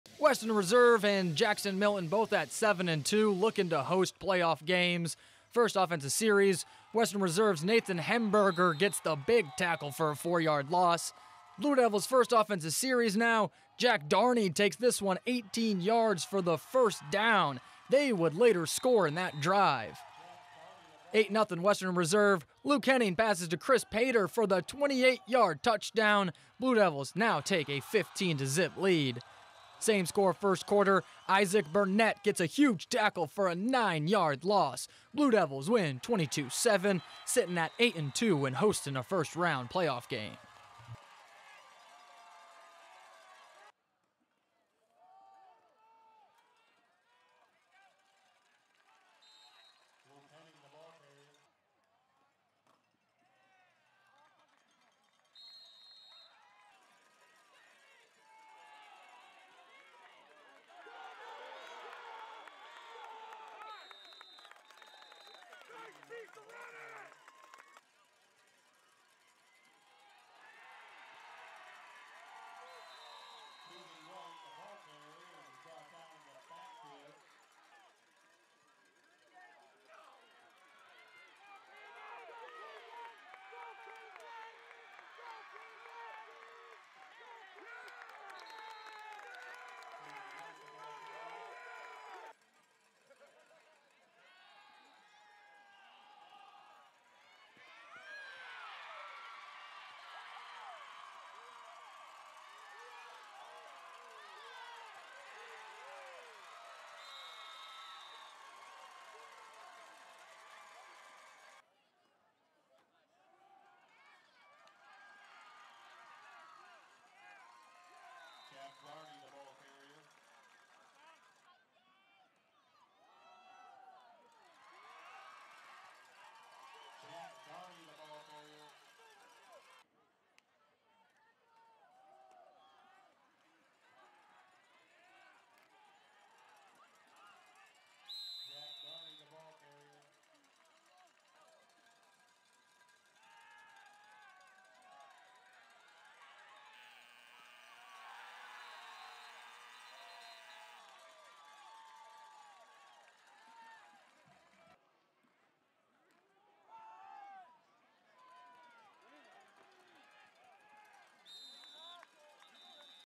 Highlights: Western Reserve vs. Jackson-Milton
Western Reserve Blue Devils vs. Jackson-Milton Blue Jays high school football game recap